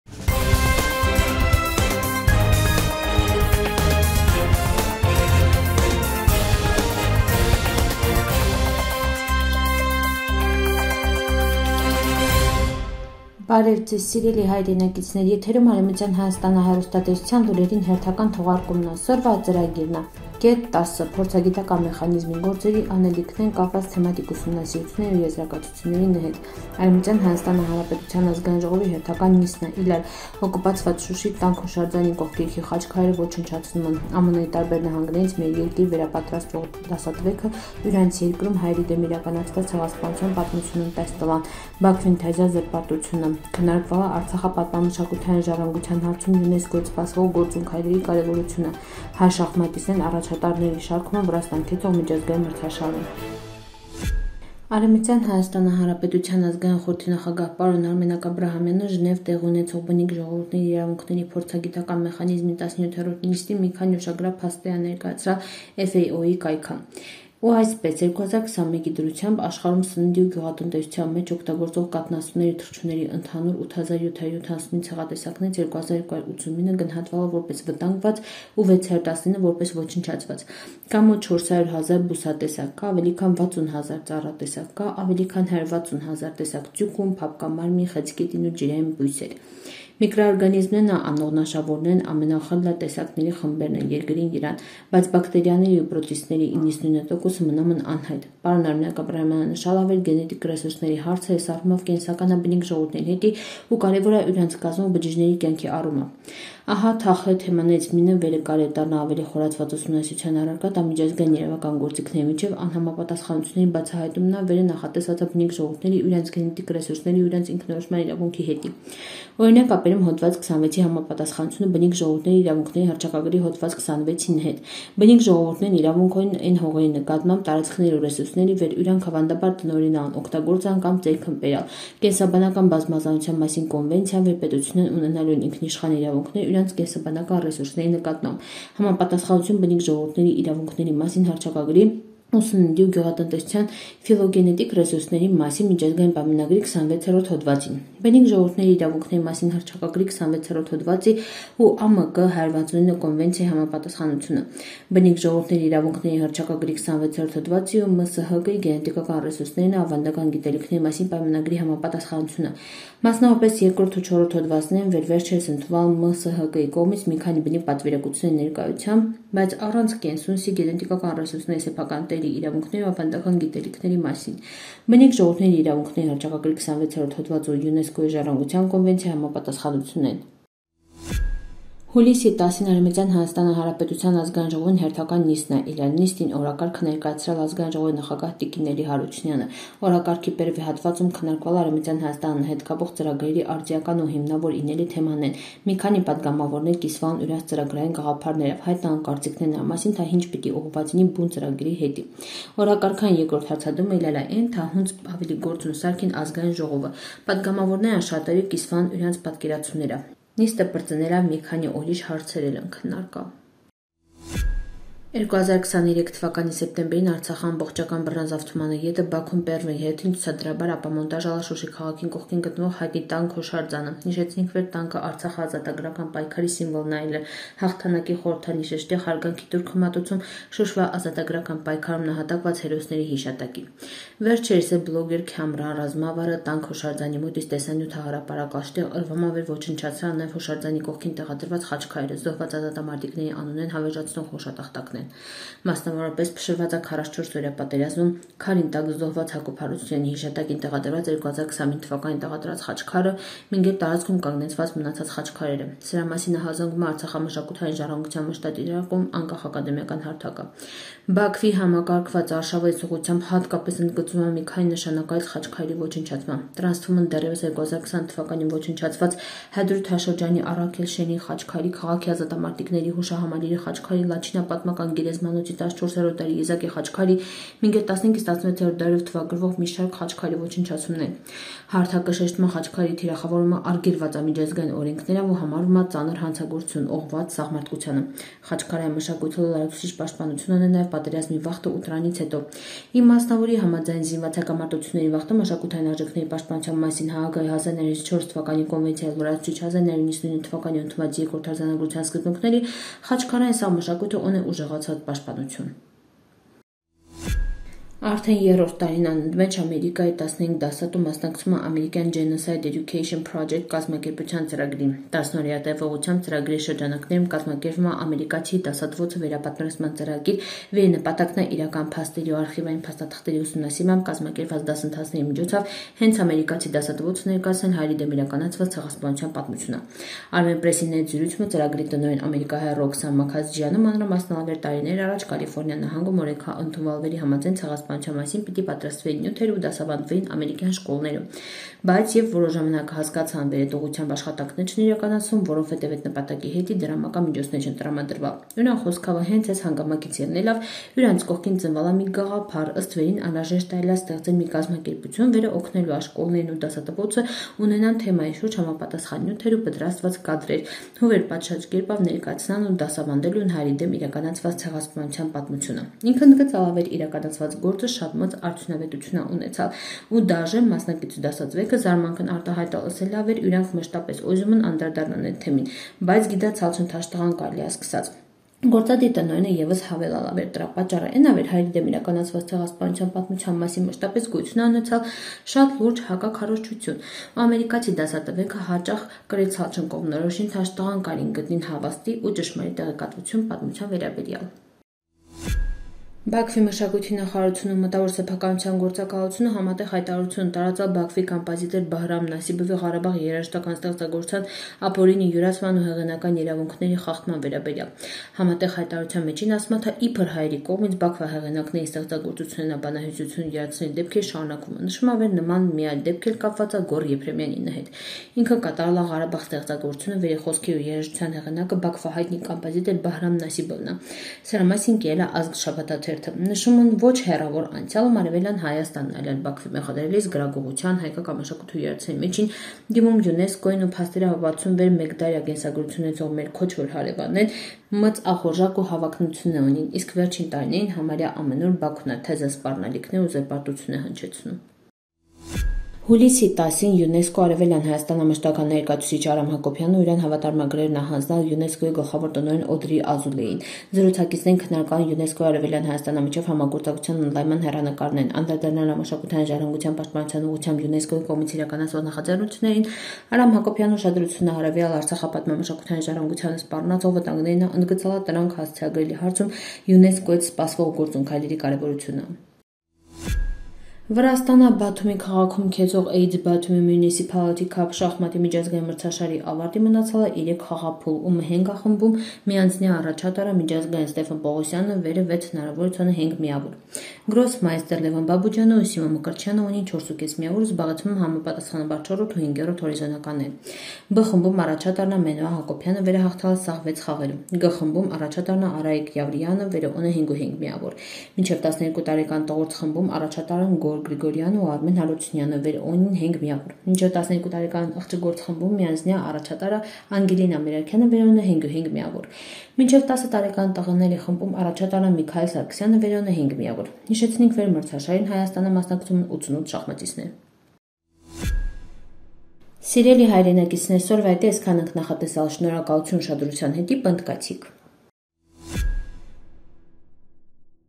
Փորձագիտական մեխանիզմի անելիքնեն ՝ կապված թեմատիկ ուսումնասիրությունների ու եզրակացությունների նհետ
1. Կետ 10: Փորձագիտական մեխանիզմի  անելիքնեն ՝ կապված թեմատիկ ուսումնասիրությունների ու եզրակացությունների նհետ
2․ ԱՀՀ Ազգային Ժողովի հերթական նիստնա իլալ
3․ Օկուպացված Շուշի Տանկ-հուշարձանին կողկերքի խաչքարերը  ոչնչացնումն
4․ ԱՄՆ-ի տարբեր նահանգներից մեր երկիր վերապատրաստվող դասատվեքը  ուրանց երկրում այերի դեմ իրականացված ցեղասպանության պատմությունն տաս տլան
5. Բաքվին թազա զրպարտությունը
6․ Քննարկվալա Արցախա պատմամշակութային ժառանգության հարցում ՅՈՒՆԵՍԿՕ-ից սպասվող գործուն քայլերի կարեւորությունը
7. Հայ շախմատիստները առաջատարների շարքումն Վրաստան քեցող  միջազգային մրցաշարում